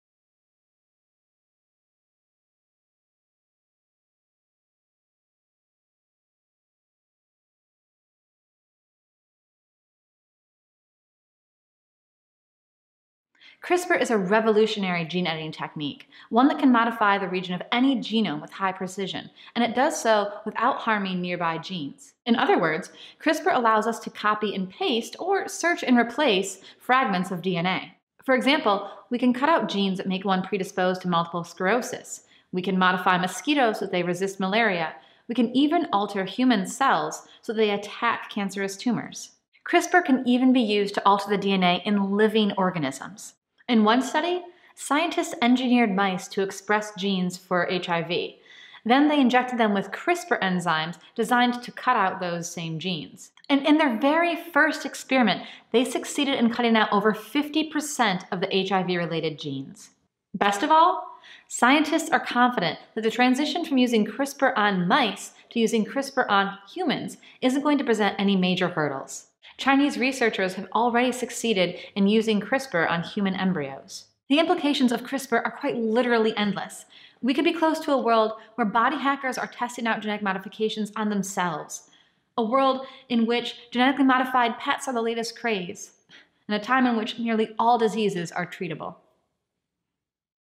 Everything You Need to Know About CRISPR in 60 Seconds
We hear a lot about CRISPR these days... but what is it, exactly?